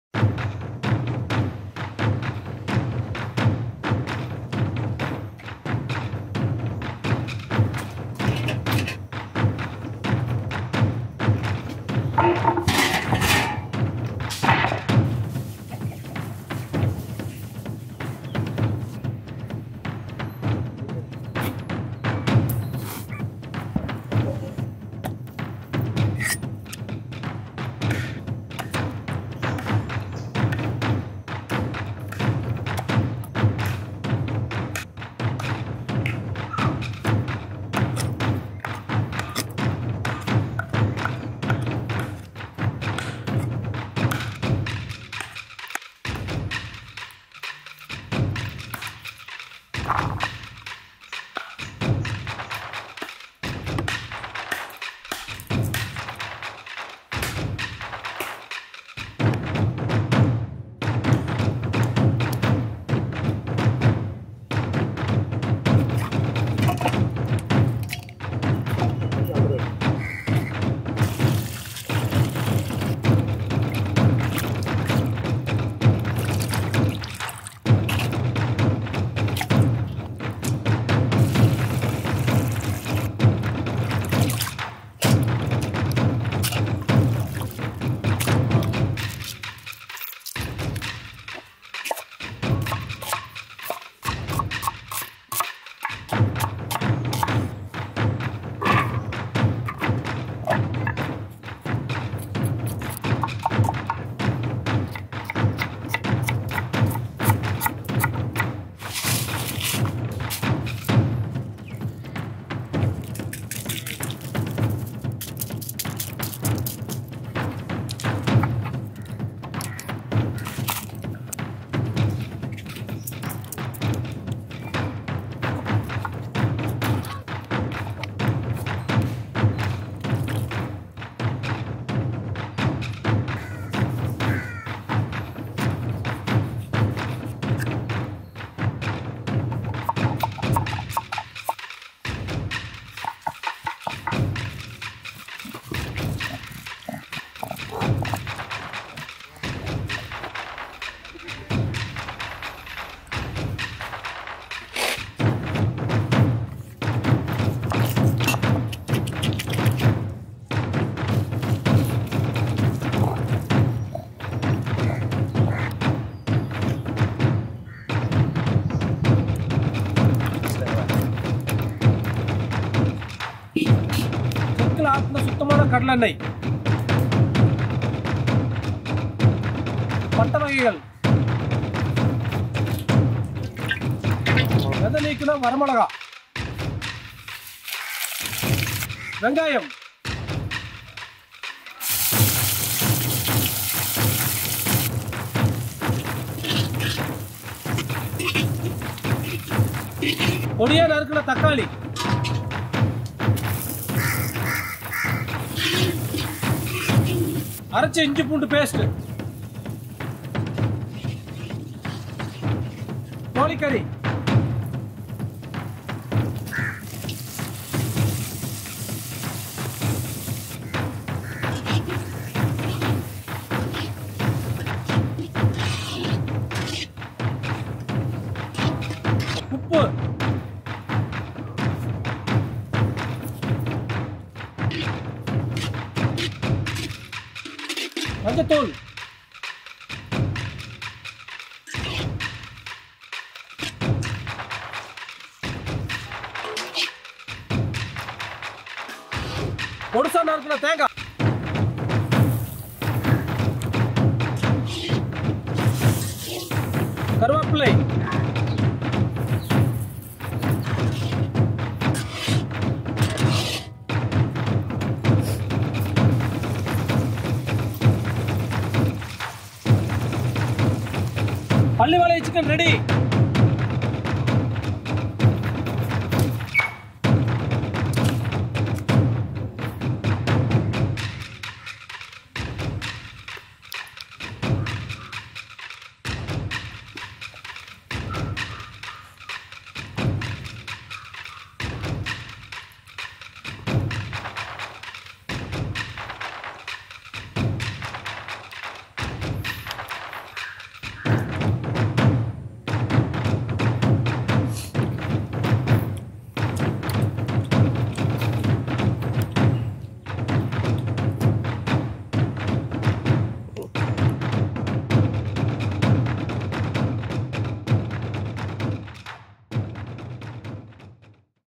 Erode Pallipalayam Chicken: Tamil Nadu's Iconic Chicken Dish | Village Cooking Masters
Hello Friends,

VCM Village cooking makes traditional village food, country foods and tasty recipes for foodies, children, villagers and orphan's.

VCM Village cooking entertains you with cooking and sharing foods.